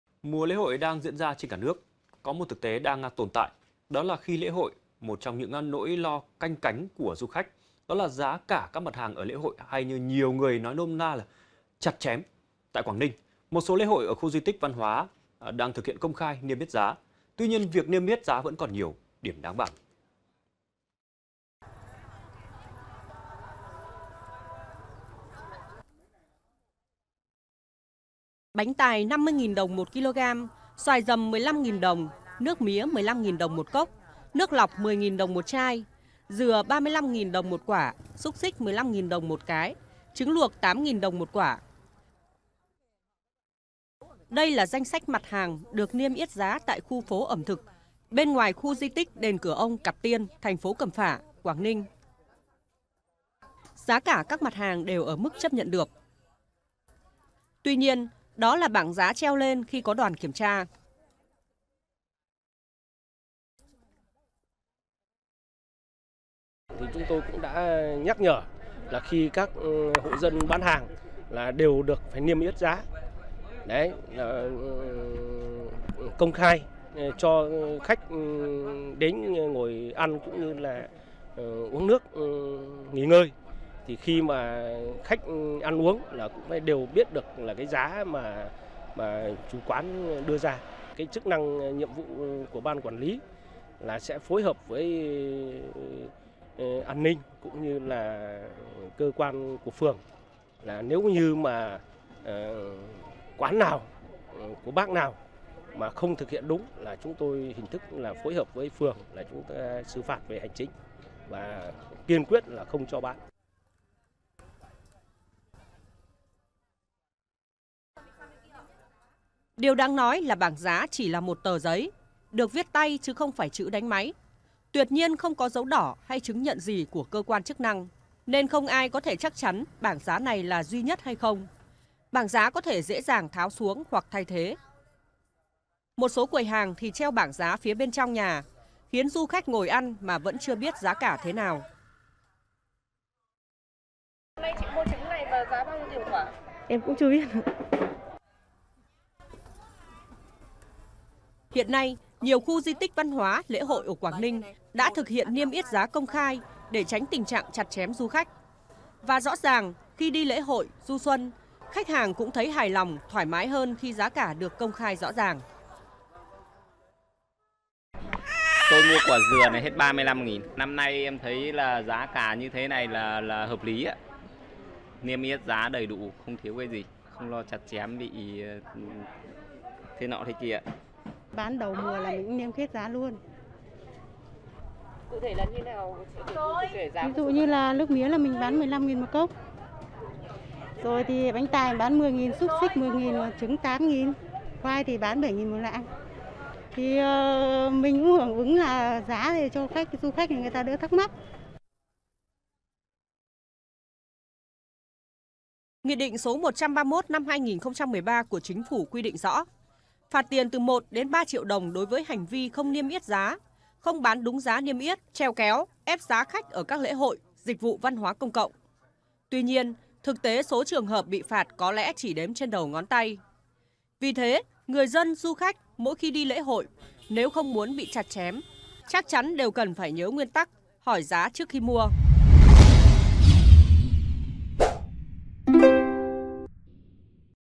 Niêm yết giá ở lễ hội: Đừng làm cho có | VTC14
VTC14 | Mùa lễ hội đang diễn ra trên cả nước. Có 1 thực tế đang tồn tại, đó là khi đi lễ hội, một trong những nỗi lo canh cánh của du khách, đó là giá cả các mặt hàng ở lễ hội, hay như nhiều người nói nôm na là “chặt chém”. Tại Quảng Ninh, một số lễ hội, khu di tích văn hóa đã thực hiện công khai, niêm yết giá. Tuy nhiên, việc niêm yết giá vẫn còn nhiều điểm đáng bàn.